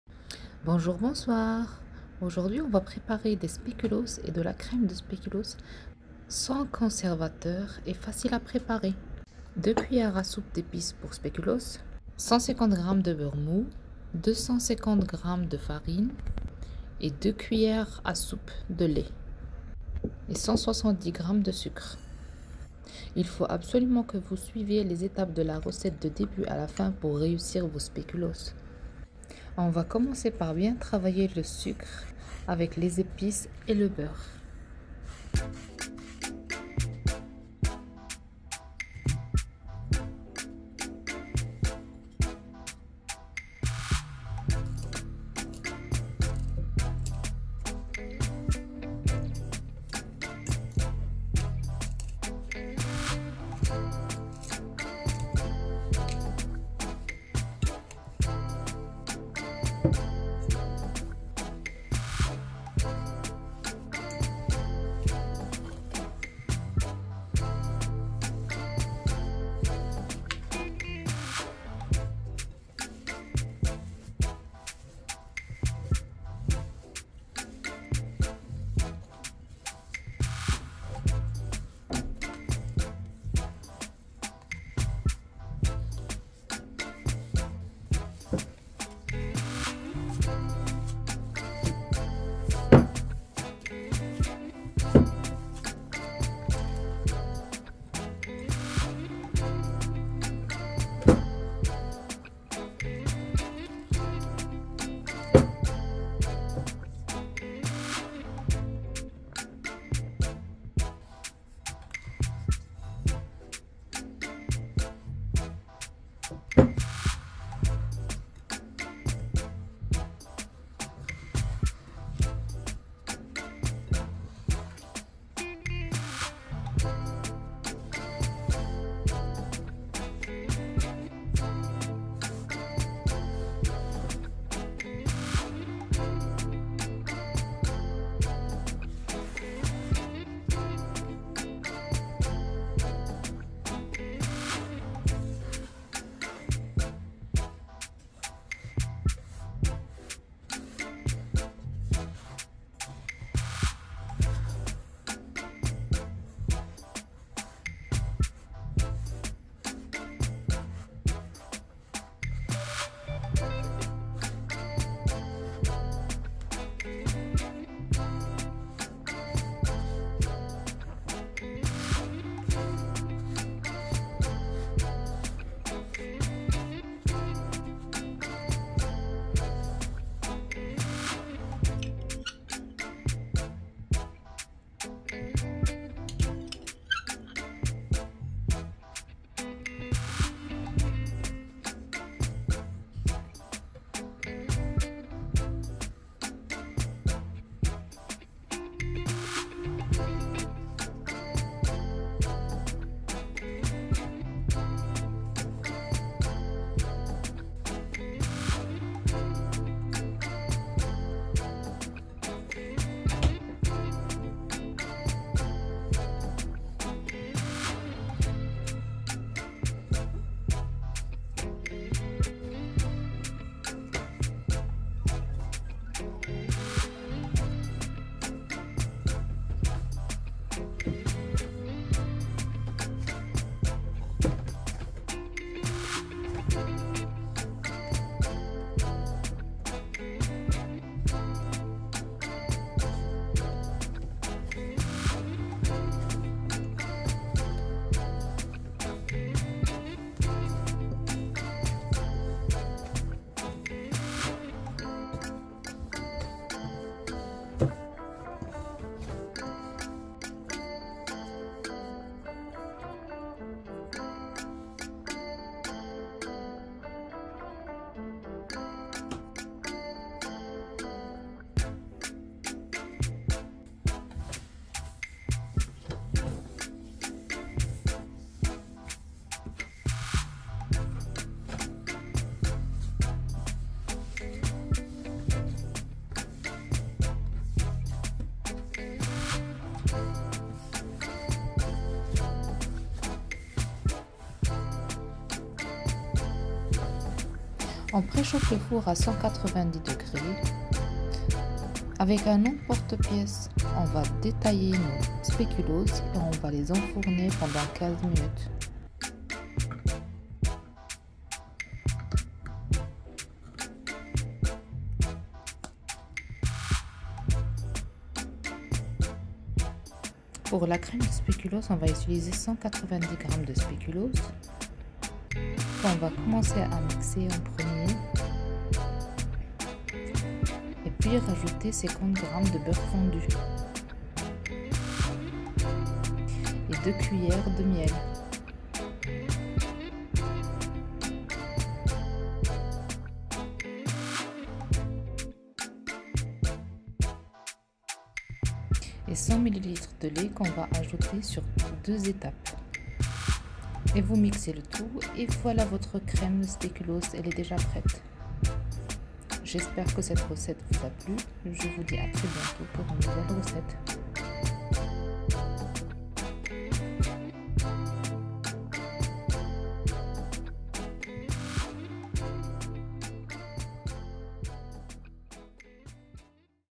Spéculoos / Crème speculoos maison, la recette facile et gourmande
Bonjour à tous ! Aujourd'hui, je vous propose une recette de spéculoos maison, des biscuits gourmands et réconfortants. Les spéculoos sont des biscuits traditionnels belges à base de cassonade, de beurre, de farine et d'épices. Ils sont généralement servis à l'occasion de la Saint-Nicolas, mais ils peuvent être dégustés tout au long de l'année.

Les spéculoos maison sont des biscuits simples à réaliser et qui plairont à tous. Ils sont parfaits pour une fin de repas ou un goûter gourmand.

Ingrédients :

170 grammes sucre brune
2 cuillères à soupe Mélange d'épices pour Spéculoos
150 grammes Beurre
250 grammes Farine
2 cuillères à soupe Lait

Crème spéculoos

180g de spéculoos
100g de lait entier
50g de beurre fondu